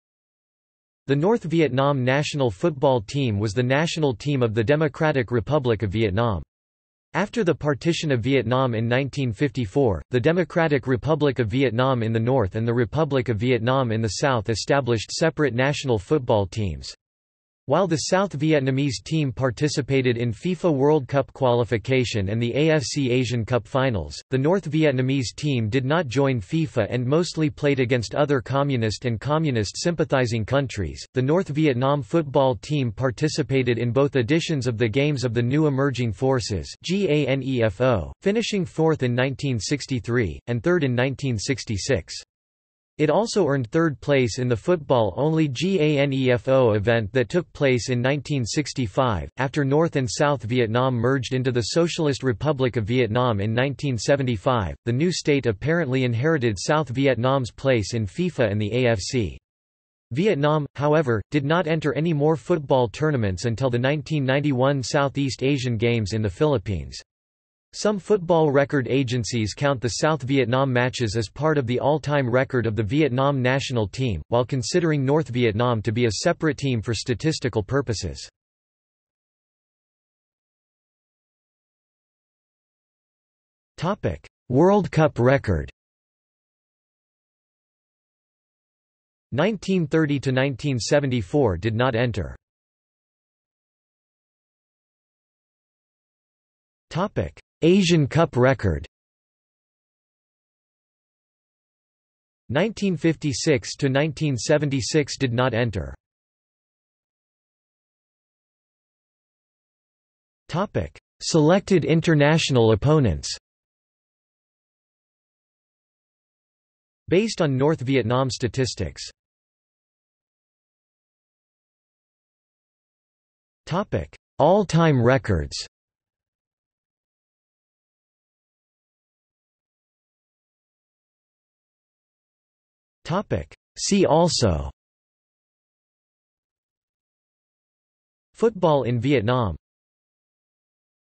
North Vietnam national football team | Wikipedia audio article
00:01:20 1 World Cup record
00:01:33 2 Asian Cup record
00:01:46 3 Selected international opponents
00:02:00 4 All-time Records
00:02:09 5 See also

- Socrates

SUMMARY
The North Vietnam national football team was the national team of the Democratic Republic of Vietnam. After the partition of Vietnam in 1954, the Democratic Republic of Vietnam in the North and the Republic of Vietnam in the South established separate national football teams. While the South Vietnamese team participated in FIFA World Cup qualification and the AFC Asian Cup finals, the North Vietnamese team did not join FIFA and mostly played against other Communist and Communist-sympathizing countries.The North Vietnam football team participated in both editions of the Games of the New Emerging Forces (GANEFO), finishing fourth in 1963, and third in 1966. It also earned third place in the football-only GANEFO event that took place in 1965.After North and South Vietnam merged into the Socialist Republic of Vietnam in 1975, the new state apparently inherited South Vietnam's place in FIFA and the AFC. Vietnam, however, did not enter any more football tournaments until the 1991 Southeast Asian Games in the Philippines. Some football record agencies count the South Vietnam matches as part of the all-time record of the Vietnam national team, while considering North Vietnam to be a separate team for statistical purposes.